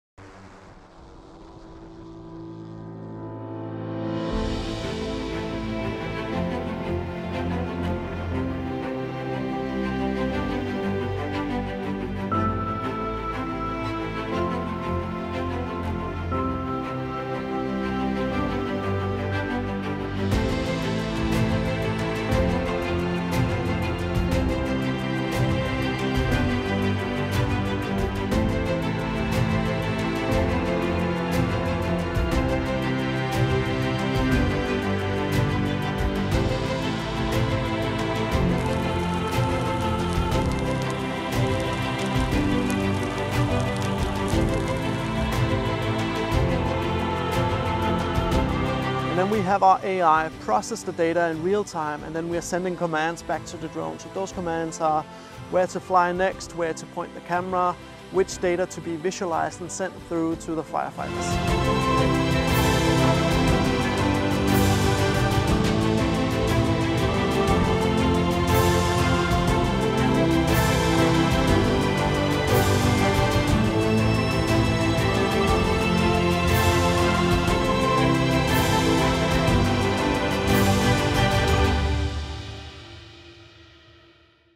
تطوير "درون" تعمل بالذكاء الاصطناعي لكبح تمدد حرائق الغابات
شركة دنماركية تطور "درون" تعمل بالذكاء الاصطناعي لكبح تمدد حرائق الغابات باستخدام كاميرا حرارية وبيانات الوقت الفعلي

لمعرفة أحدث المستجدات.. عبر البث المباشر من هنا ⤵
وبإمكانكم متابعة العربية على منصات التواصل